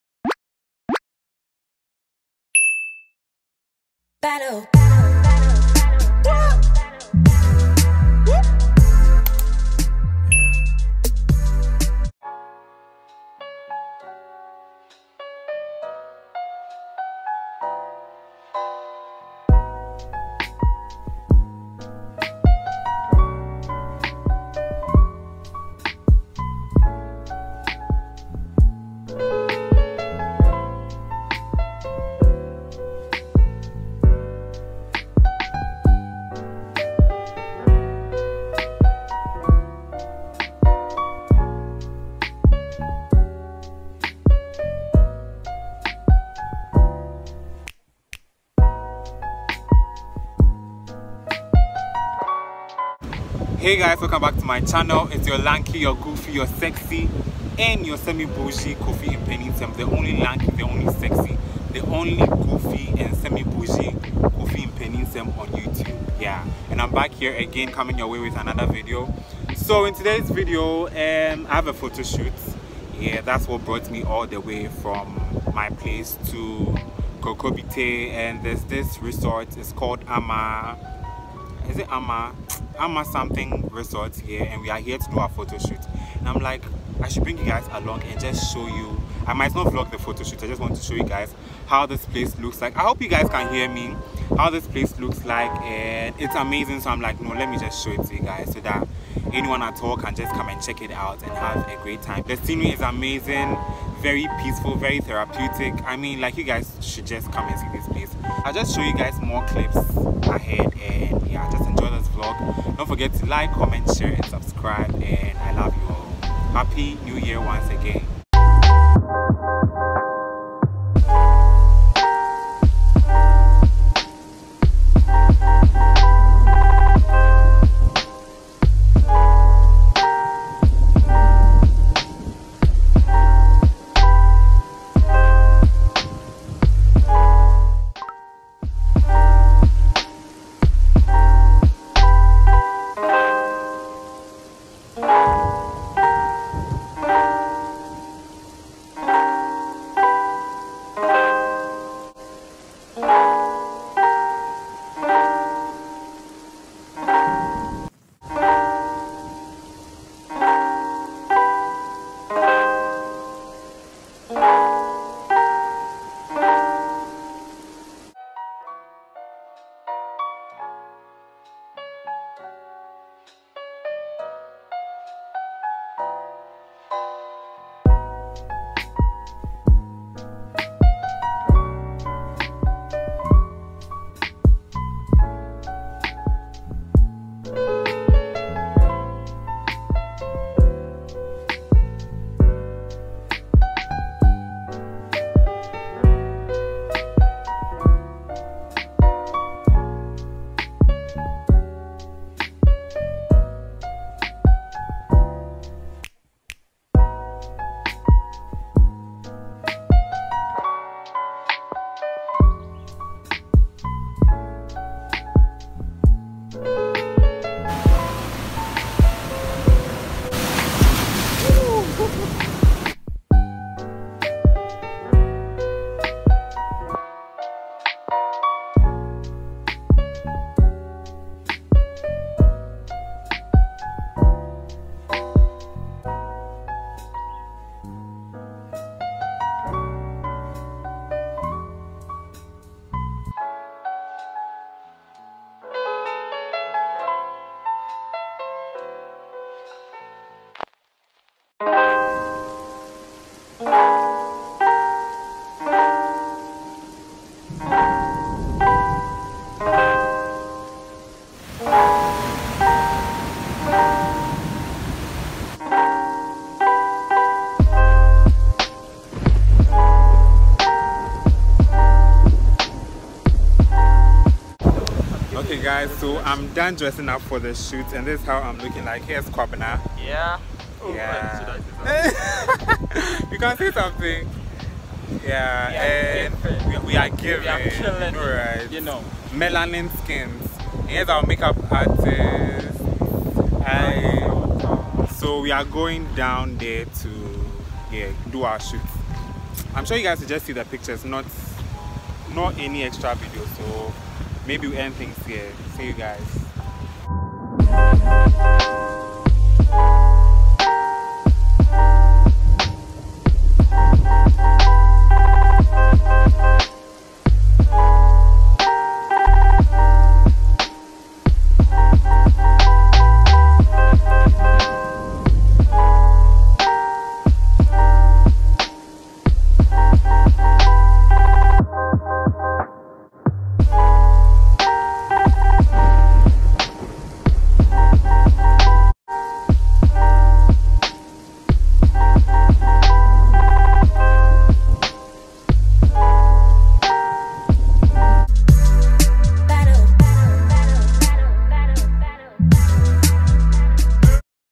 Discovered a hidden beach In Ghana 🏖
Hey there guys, it’s another year with another brand new video!!! Thanks for being part of my journey, enjoy this video!